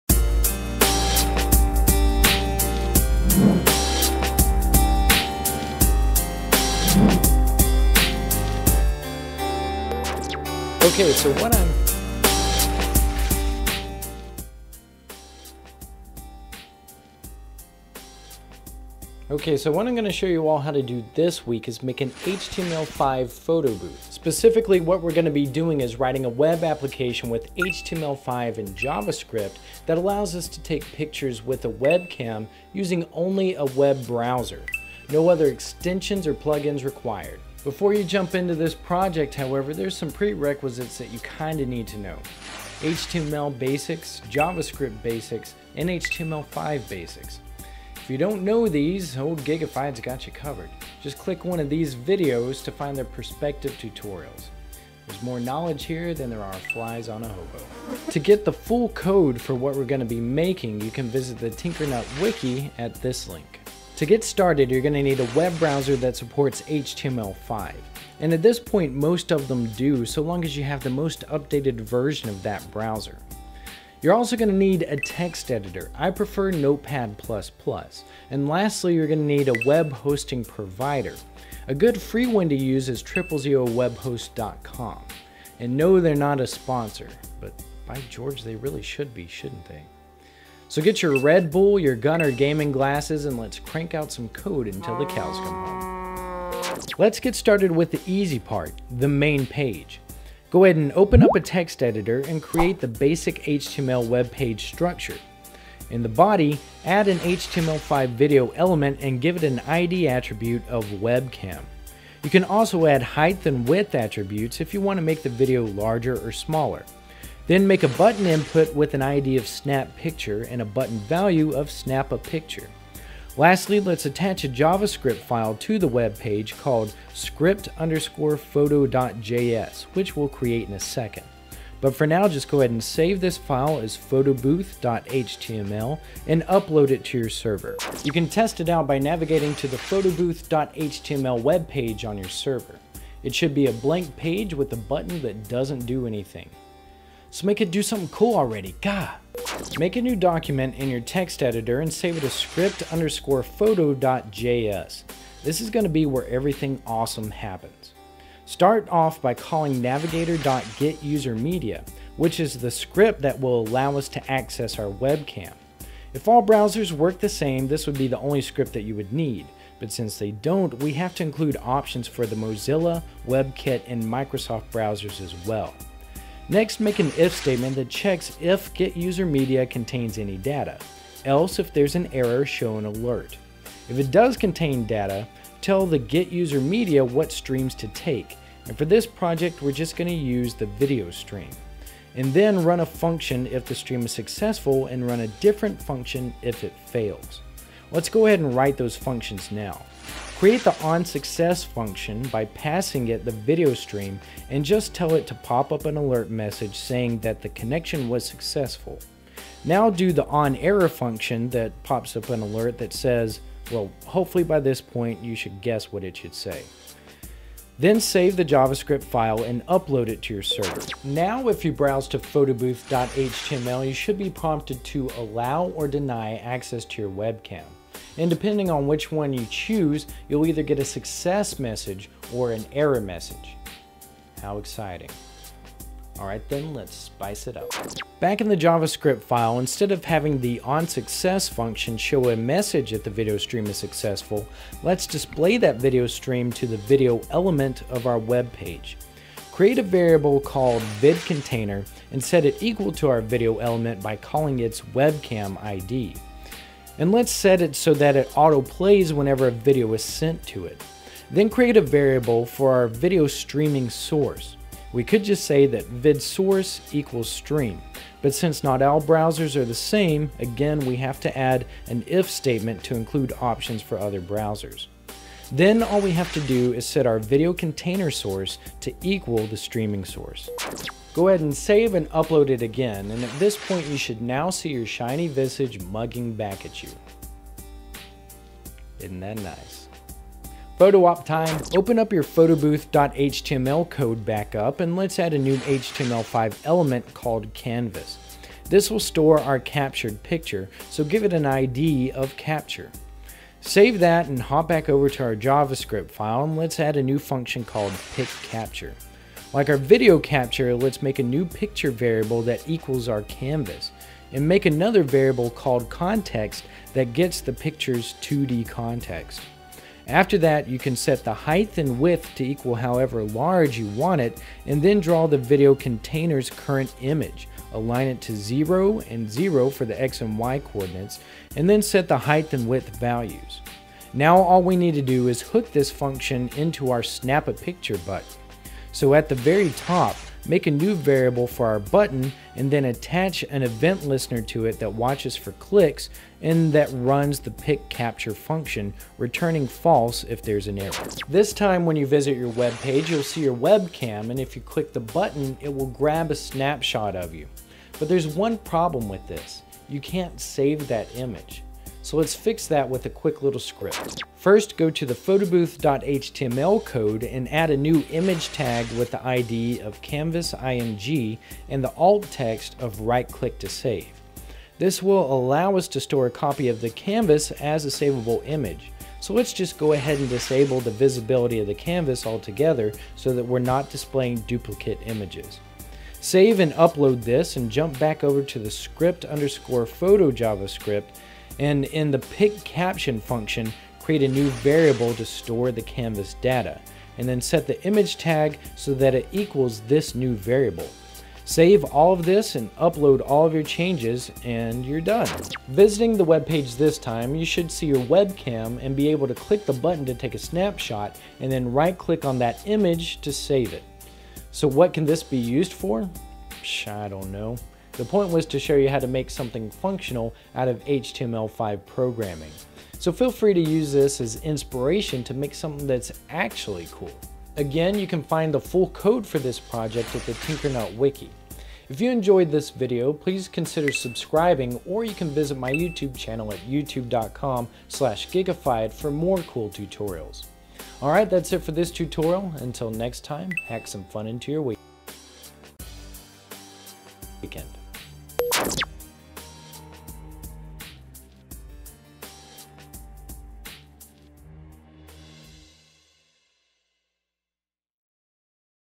Programming Tip: Make A Photo Booth Web App!
HTML5 is ramping up to become a more and more robust markup language. So flex your code monkey muscles and lets make an HTML5 photo booth application.